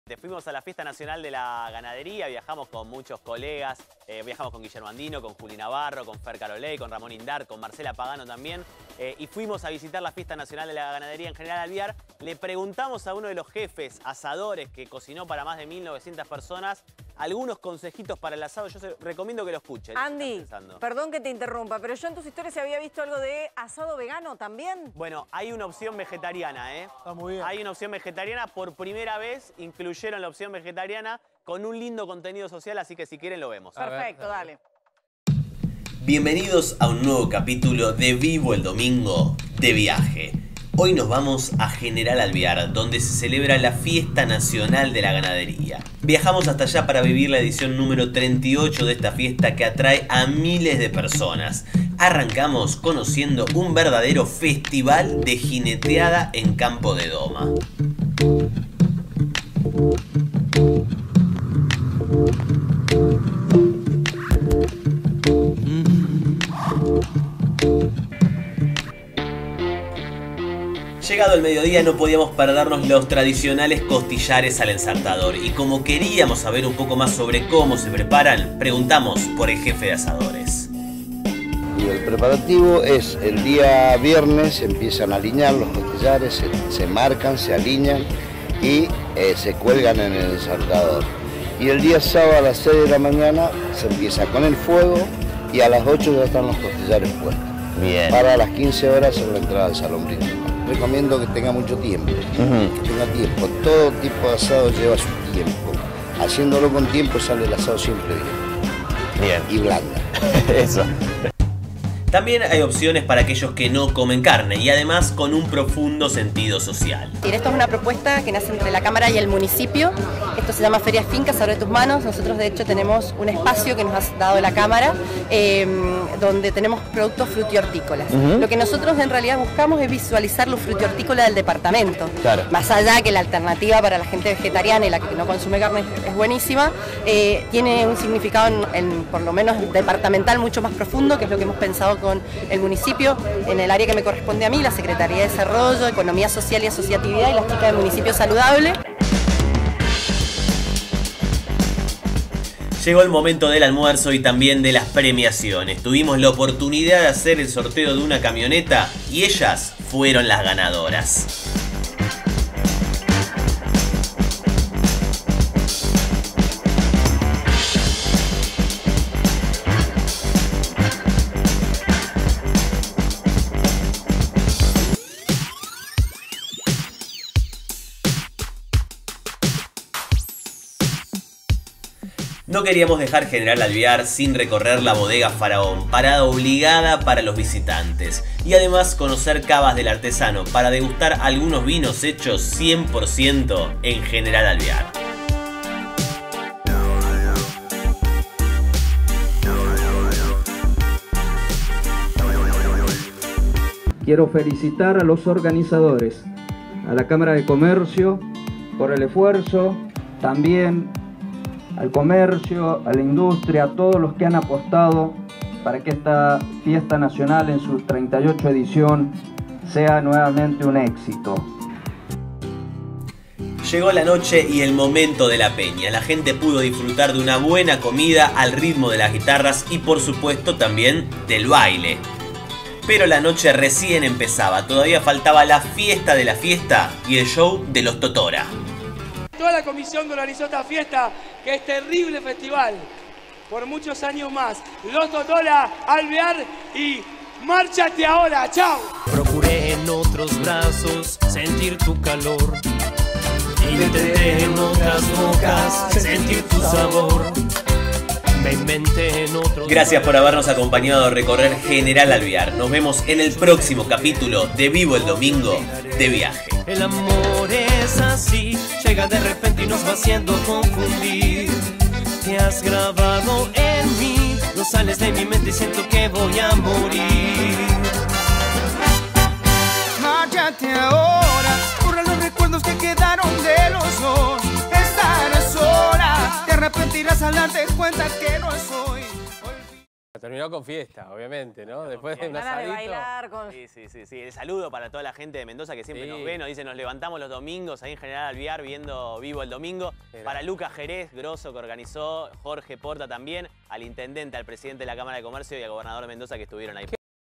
Mendoza: ¿Cómo es la Fiesta de la Ganadería? Consejos para el asado
Viajamos a General Alvear en Mendoza para vivir la Fiesta Nacional de la Ganadería. Secretos para hacer un buen asado, opciones vegetarianas y mucho más.